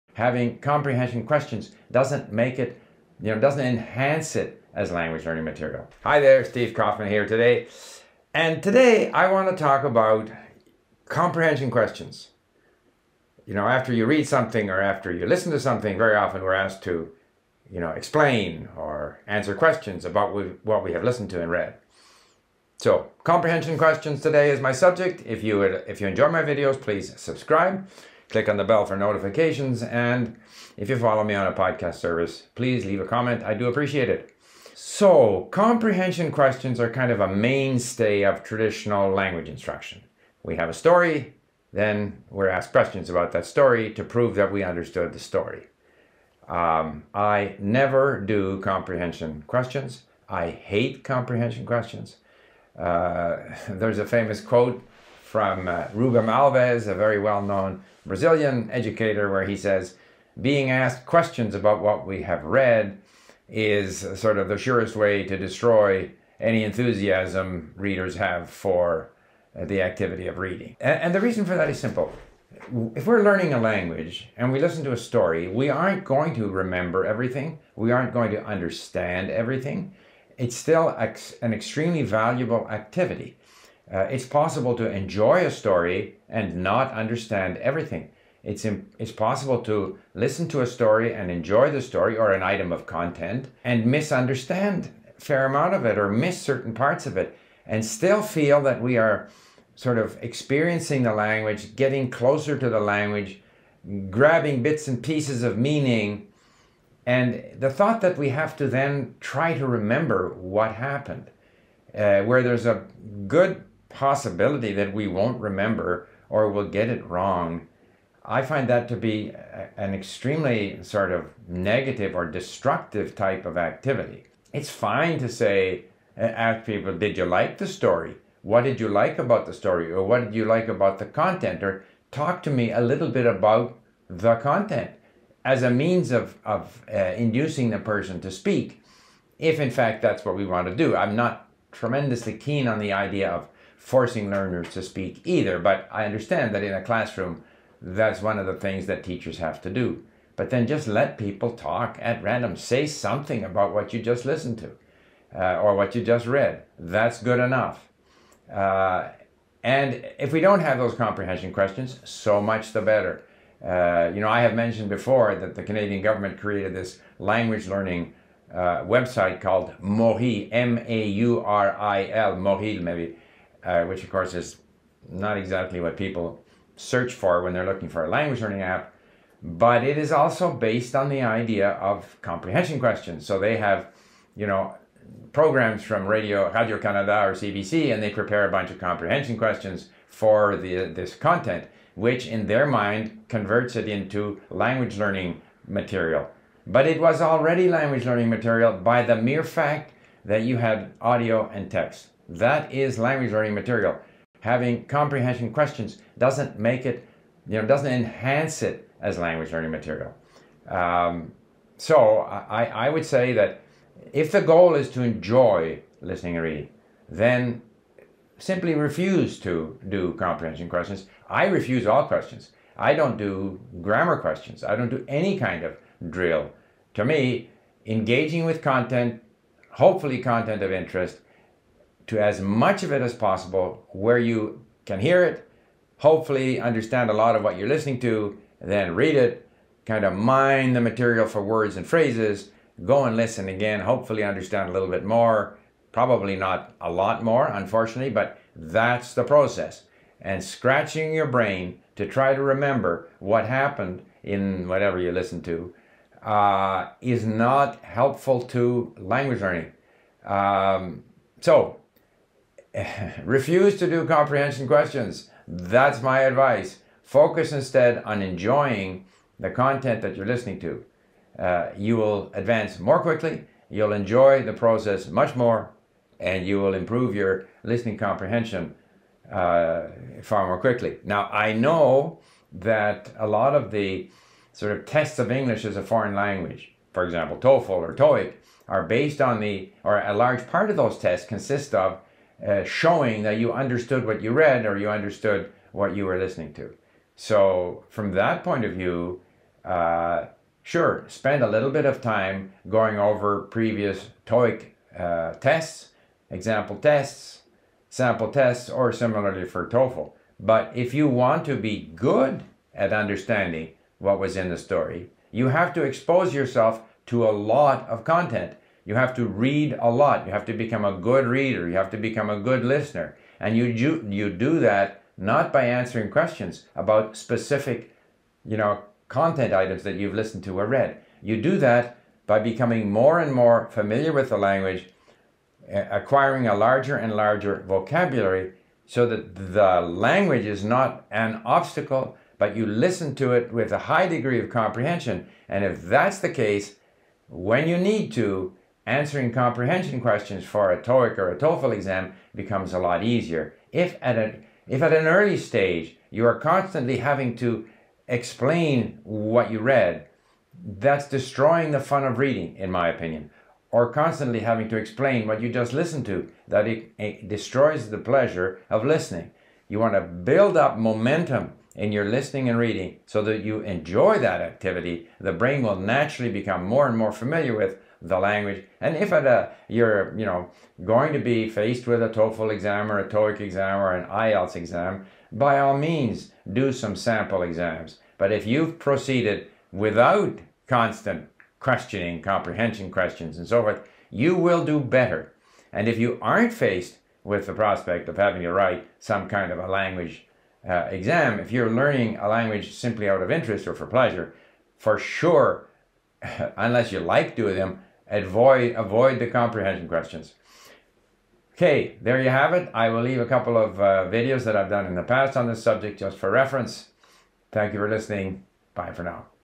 One Thing I AVOID When Learning a Language
CC subtitles available in English.

Do you enjoy answering comprehension questions about content in your target language? Many teachers and writers of language learning textbooks seem to think these questions are vital. I couldn’t disagree more.

0:00 Why I don't do comprehension questions when learning a language.
2:05 The types of questions that are fine to ask regarding content in the target language.
2:44 The Canadian government's French learning app.
4:44 When comprehension questions are unavoidable: TOEFL, TOEIC, IELTS, etc.